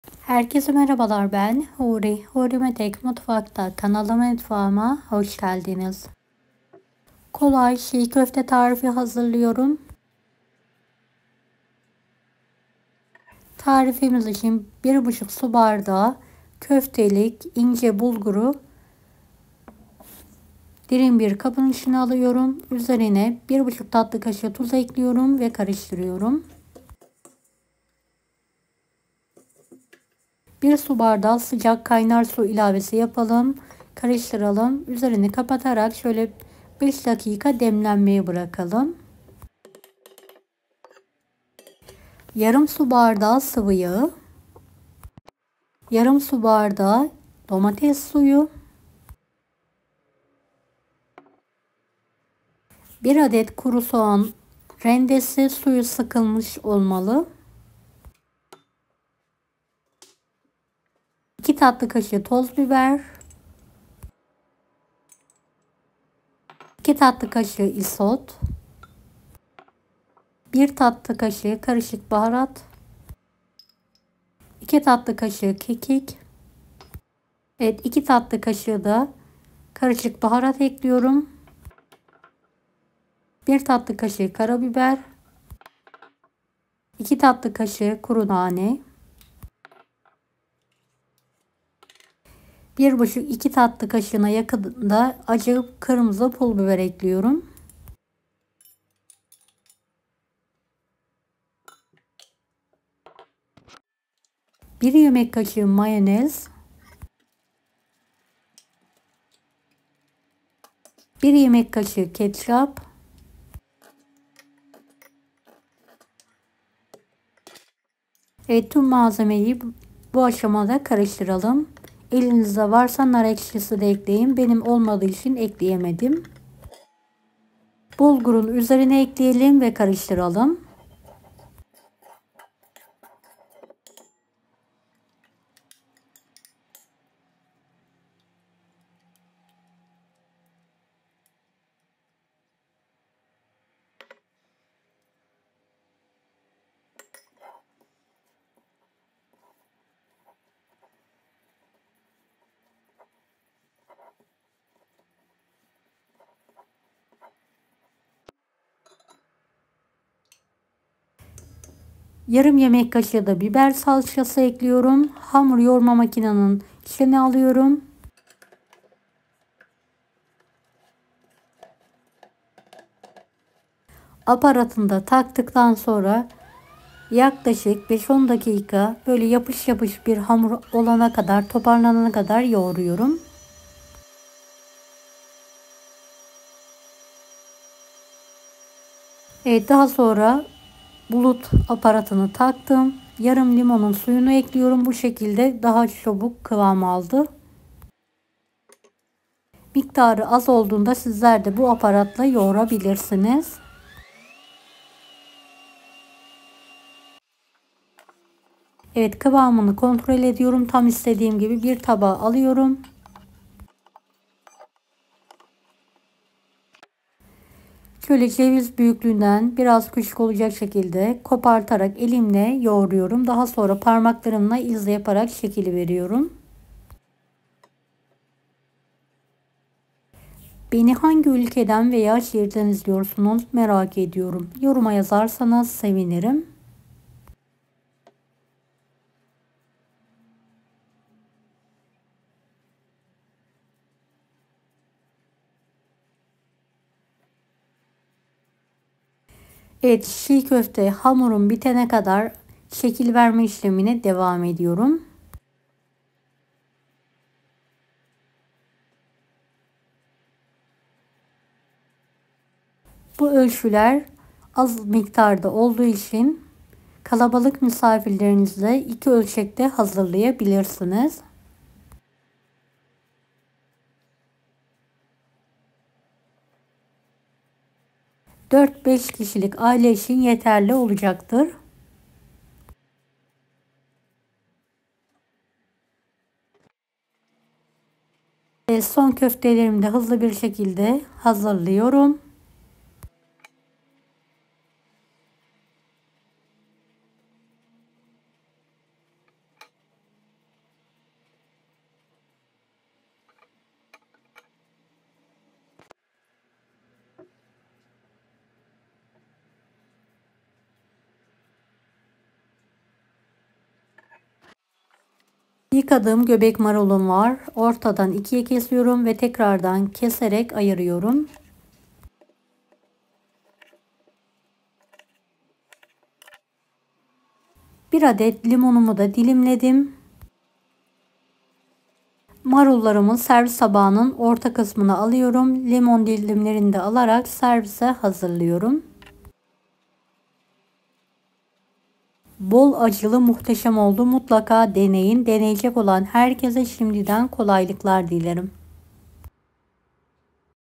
10 DAKİKADA Ekonomik ÇİĞ KÖFTE Tarifi. 1,5 BUÇUK SU Bardağı BULGURLA Muhteşem ÇİĞ Köfte
10 DAKİKADA EKONOMİK ÇİĞK KÖFTE TARİFİ. 1,5 BUÇUK SU BARDAĞI BULGURLA MUHTEŞEM ÇİĞ KÖFTE TARİFİ.

MALZEMELER:

1,5 BUÇUK SU BARDAĞI İNCE BULGUR
1,5 TATLI KAŞIĞI TUZ
1 SU BARDAĞI KAYNAR SICAK SU
YARIM SU BARDAĞI SIVIYAĞI
YARIM SU BARDAĞI DOMATES SUYU
1 KURU SOĞAN RENDESİ SUYU SIKILMIŞ OLMALI
1 YEMEK KAŞIĞI MAYONZE
1 YEMEK KAŞIĞI KETÇAP
2 TATLI KAŞIĞI ACI PUL BİBER
2 TATLI KAŞIĞI NANE
2 TATLI KAŞIĞI KEKİK
1 TATLI KAŞIĞI KARABİBER
1 TATLI KAŞIĞI KARIŞIK BAHARAT
2 TATLI KAŞIĞI FARKLI KARIŞIK BAHARAT
1 YEMEK KAŞIĞI BİBER SALÇASI
1 LİMONUN SUYU
NAR EKSİSİ

GÖBEK MARUL
LİMON DİLİMLERİ